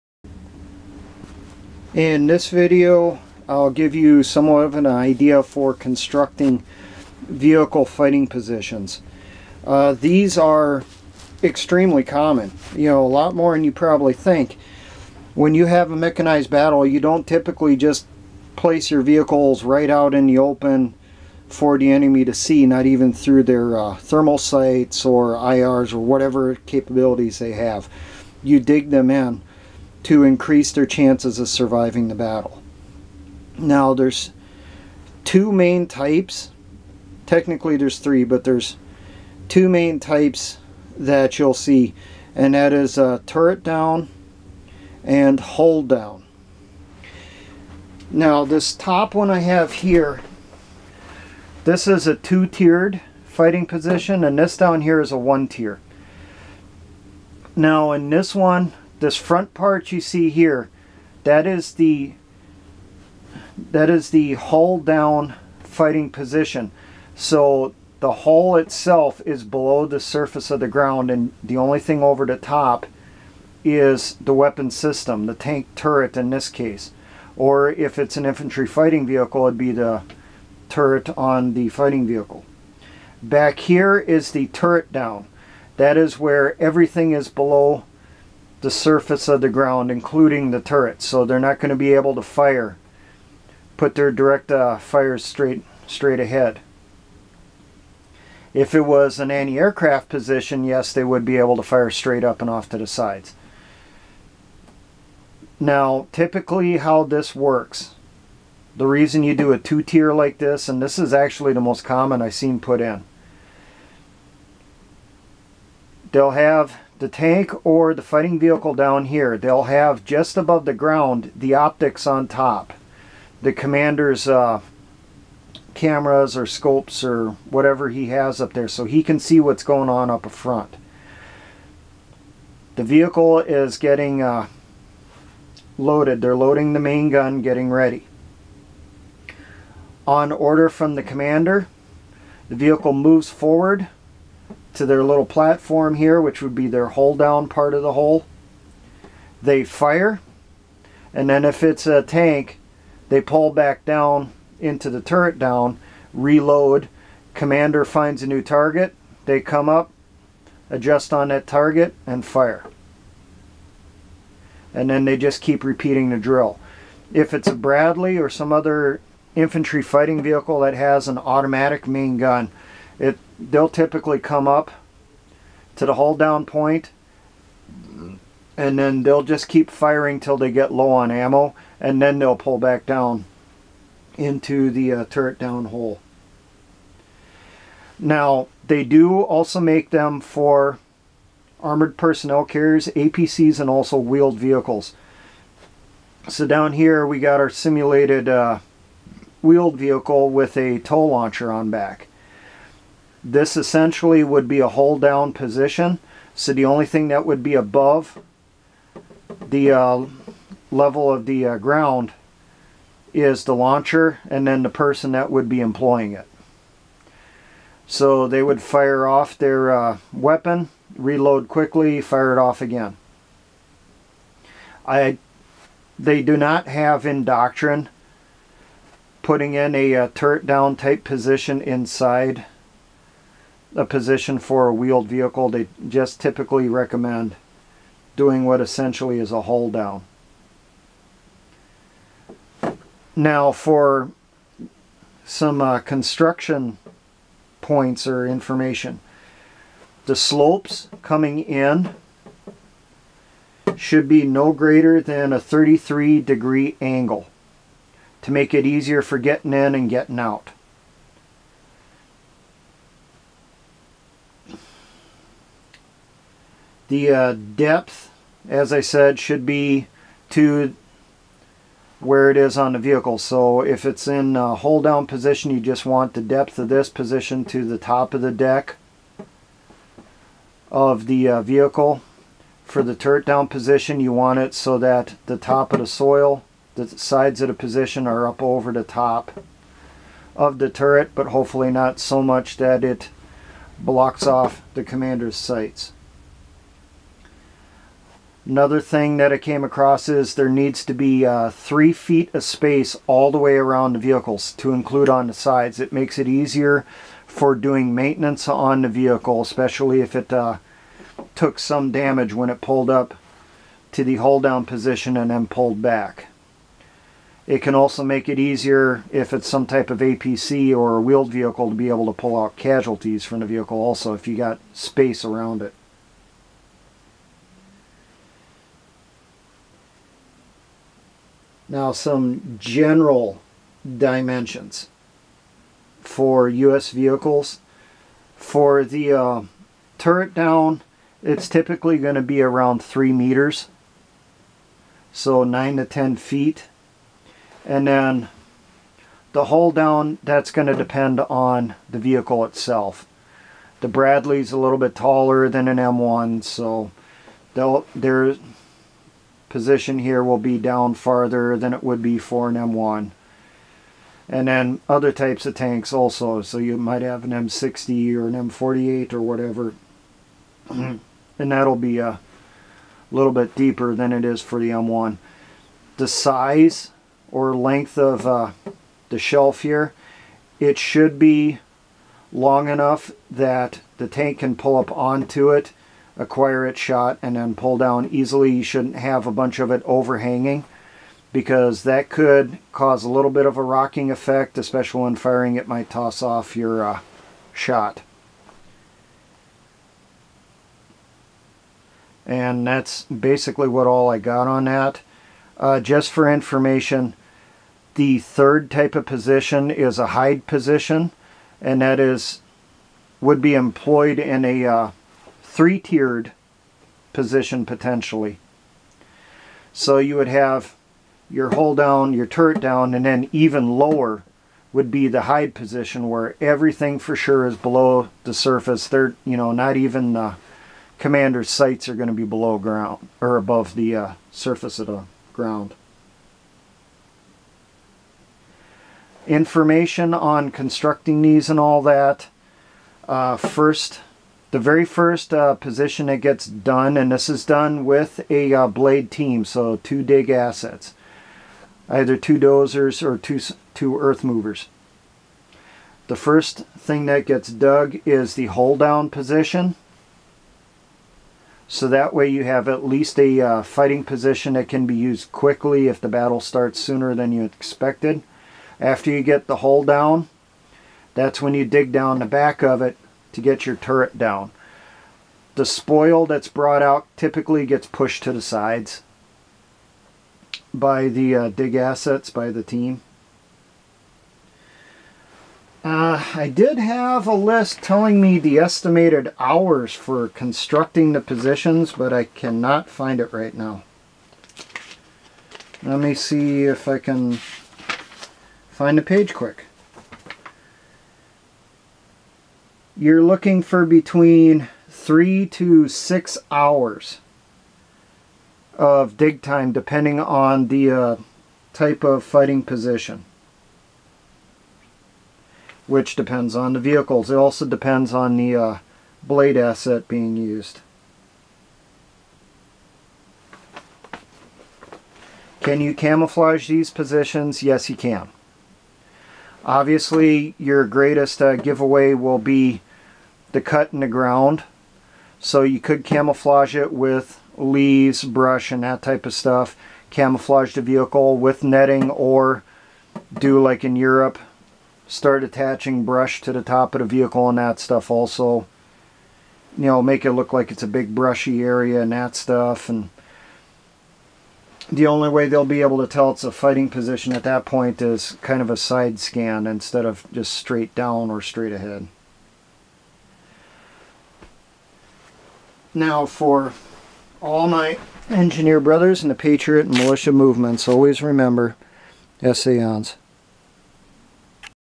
Militia Engineer Tasks- Constructing Vehicle Fighting Positions
This video gives some ideas on constructing vehicle positions for defending an area.  The dimensions of the position depends on the vehicle that is going to fight from it.  Gets specifics from FM 5-34 Engineer Field Data.